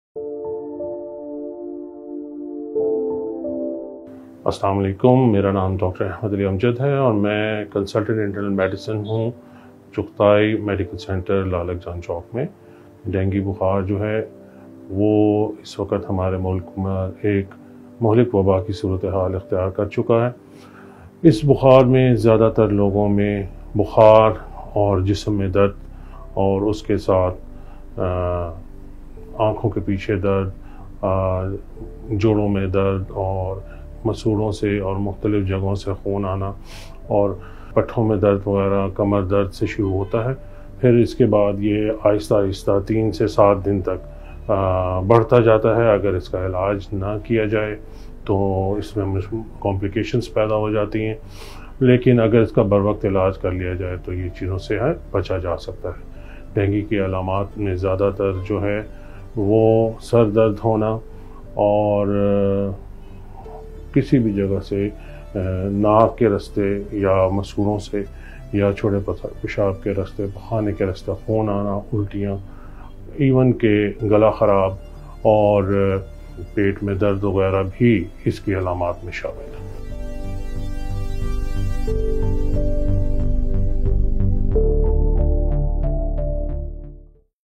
Dengue virus symptoms video
Dengue virus symptoms!
Listen to Dr. Ahmed Ali Amjad, Consultant INT. Medicine, as he explains dengue virus symptoms.
It is important to understand dengue virus symptoms to ensure timely treatment.